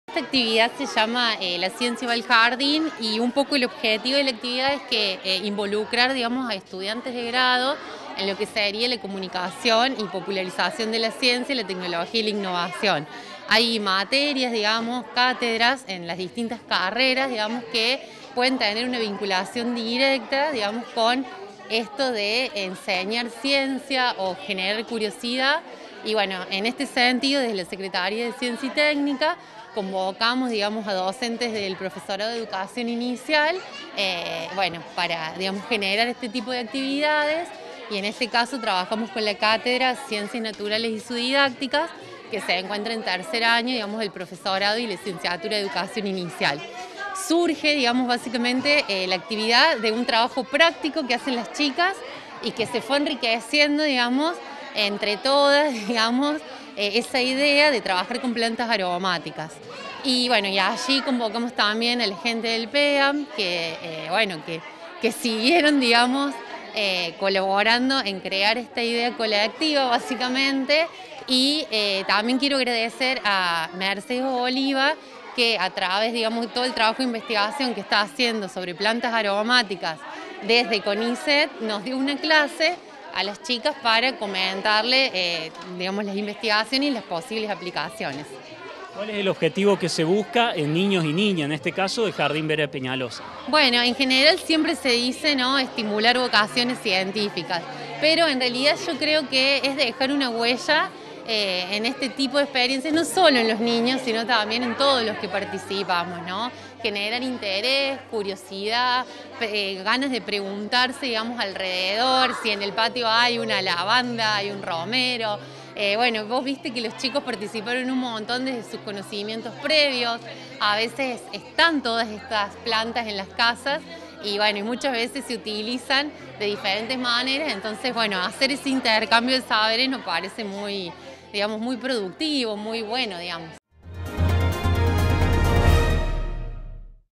LA CIENCIA VA AL JARDÍN: NIÑOS DEL ROSARIO VERA PEÑALOZA TRABAJARON CON PLANTAS AROMÁTICAS
FERNANDA MELGAR – PROGRAMA COMUNICACIÓN PÚBLICA DE LA CIENCIA